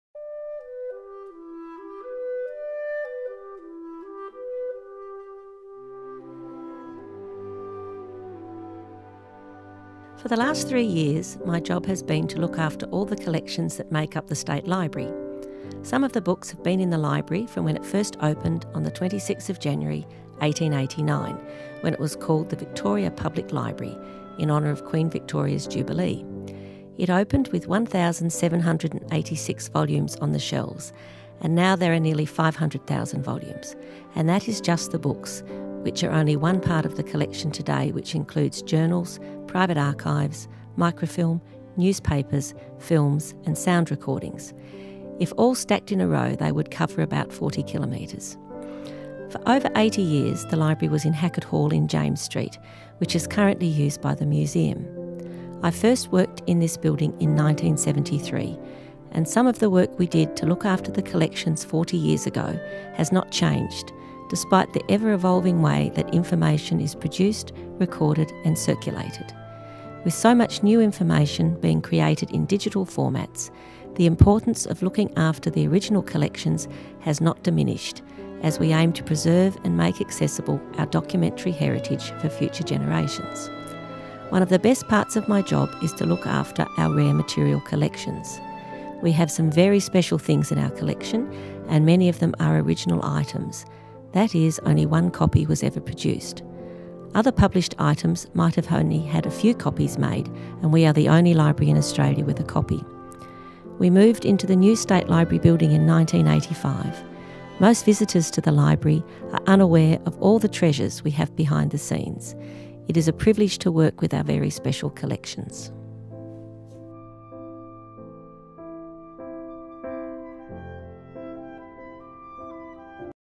The Toni Young Story – Shelf Life
Watch this story to learn about working with the collections at the State Library of Western Australia.
This Digital Story is part of a series called Shelf Life. The series goes behind the scenes and investigates some of the different jobs around the State Library of Western Australia. They were created to help celebrate the State Library’s 125th anniversary in 2014.
Story and narration: Toni Young
Animation: Helvio Machado
Shelf Life was a collaborative project between the State Library of Western Australia and the Central Institute of Technology.